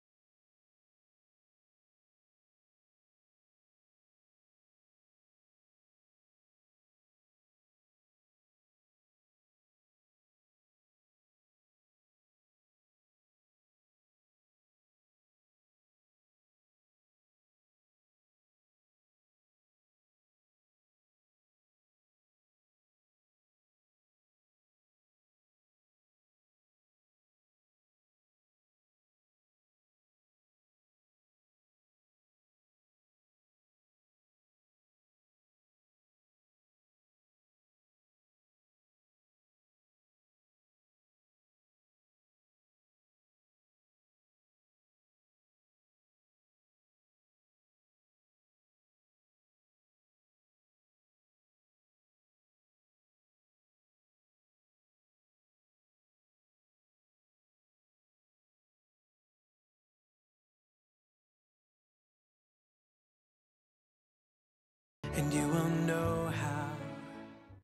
My 2015 Toronto YSA FERP Invite
Macy, I made this little video for you as my way of inviting you to FERP.
I didn't expect you to come back to Toronto but I wanted to make this invite for you even if you were in New York.
I know you've got a lot going on this weekend and you want to spend time with your family as well.

This was just more to let you know that you're worth doing something a little special for, and if you do have a few hours to spare, I'd love if you came to FERP with me as my date.